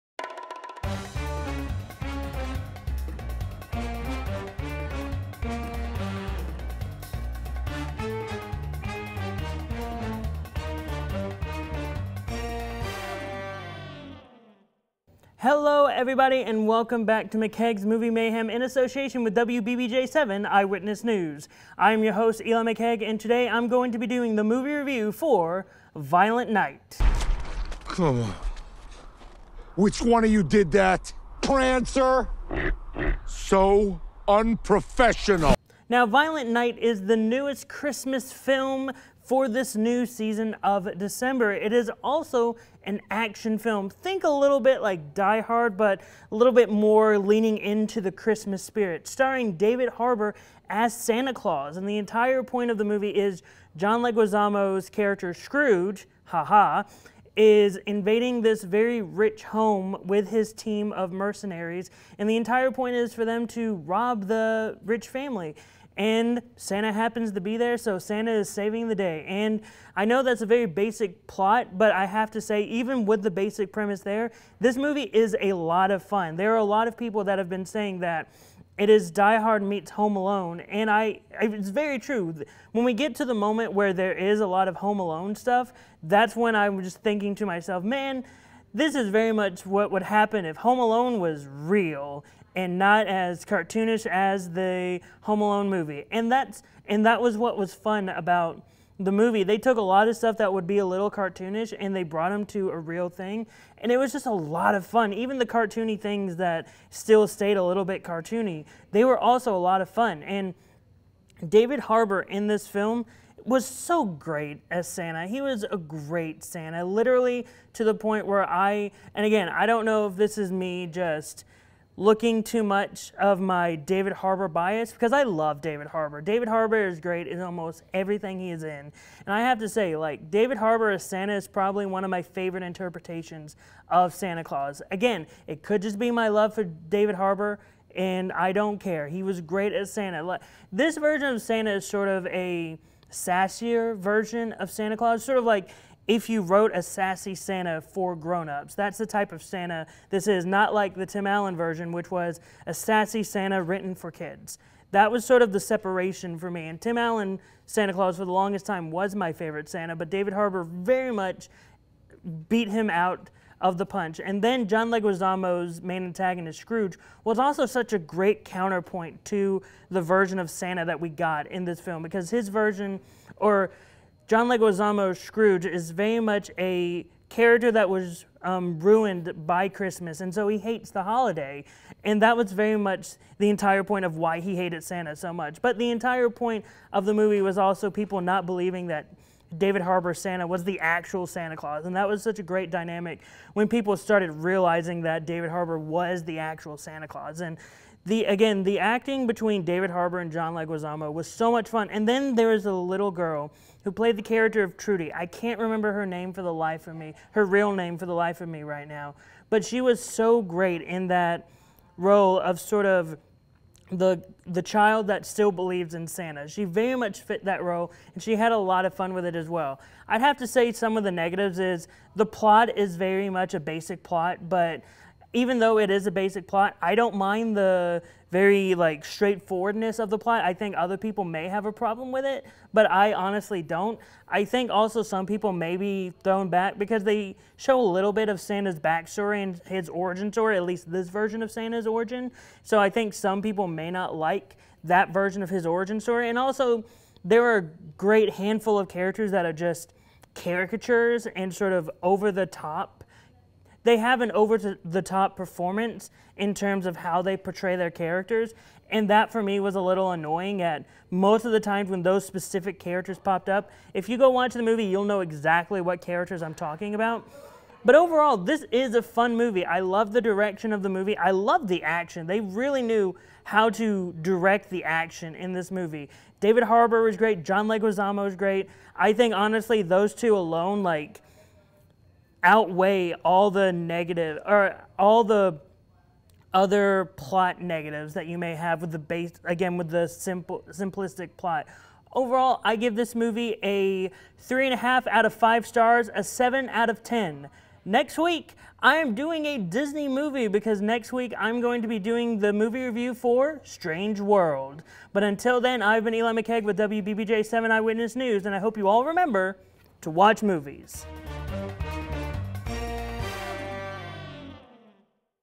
McCaig's Movie Mayhem: Violent Night Review
WBBJ 7 Eyewitness News' very own Eli McCaig is here with a review of Universal Pictures' Violent Night.

An elite team of mercenaries breaks into a family compound on Christmas Eve, taking everyone hostage inside. However, they aren't prepared for a surprise combatant: Santa Claus is on the grounds, and he's about to show why this Nick is no saint.

The movie is available in theaters.